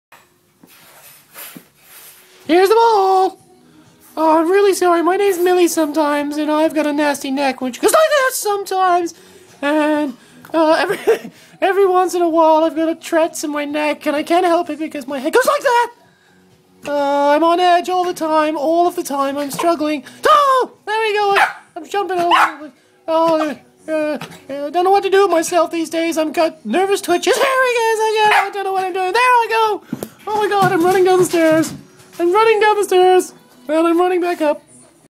Dig with trets
My dog mily has trets syndrome. On edge all the time!